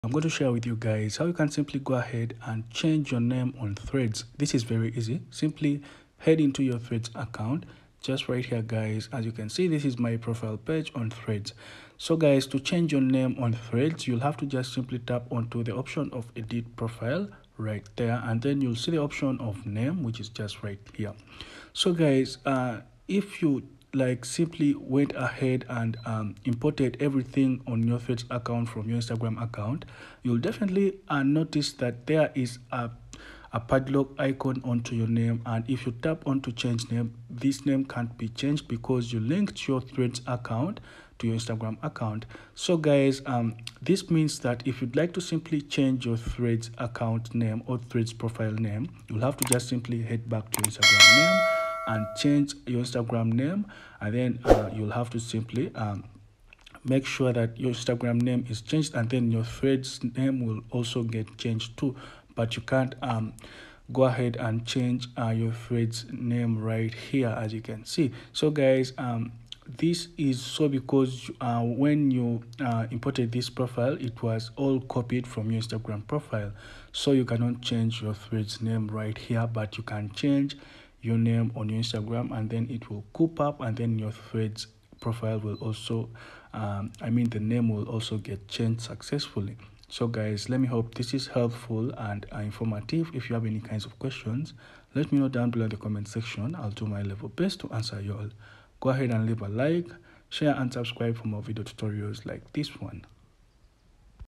How To Change Name On Threads Without your Instagram name Not Changing | Simple Tutorial
In this video tutorial we show you how you can change to any other name on threads without affecting your Instagram user name